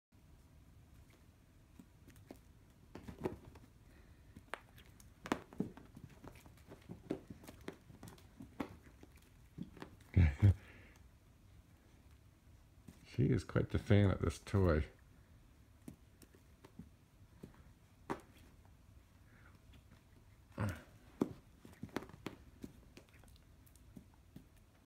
Entertaining my Mainecoon
Gypsy sure loves the cat catcher toy